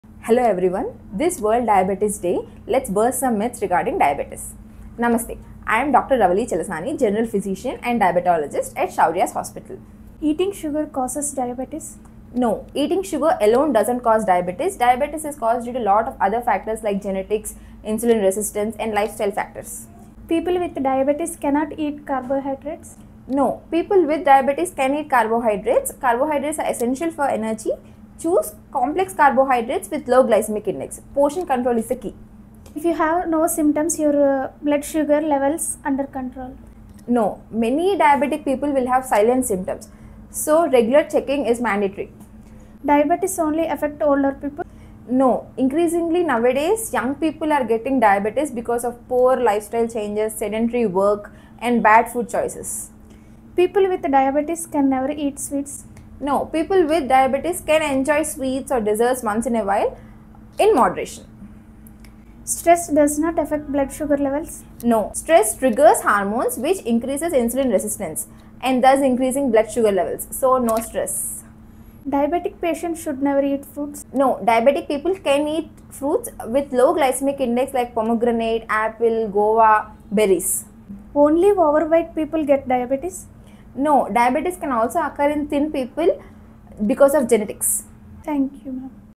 DIABETES - MYTHS❌ vs FACTS ✅
Let's bust some common diabetes myths before they mess with your health.
Got a myth you've heard and want me to bust?